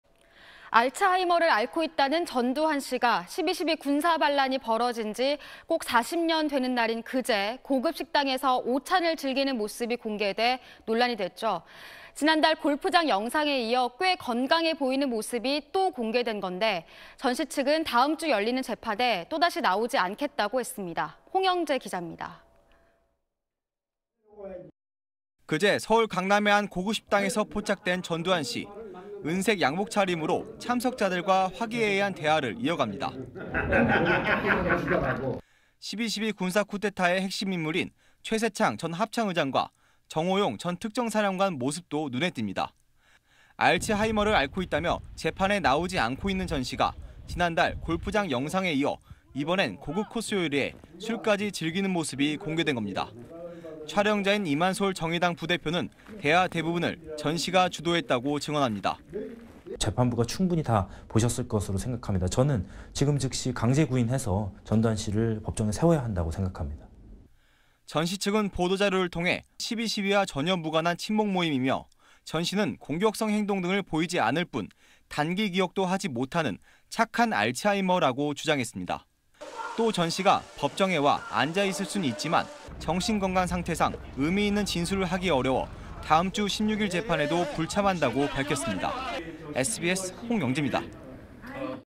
'골프·쿠데타 자축'은 하면서…전두환, 재판 또 불출석 / SBS
알츠하이머를 앓고 있다는 전두환 씨가 12.12 군사반란이 벌어진 지 꼭 40년 되는 날인 그제(12일) 고급 식당에서 오찬을 즐기는 모습이 공개돼 논란이 됐죠. 지난달 골프장 영상에 이어 꽤 건강해 보이는 모습이 또 공개된 것인데, 전 씨 측은 다음 주 열리는 재판에 또다시 나오지 않겠다고 했습니다.
홍영재 기자입니다.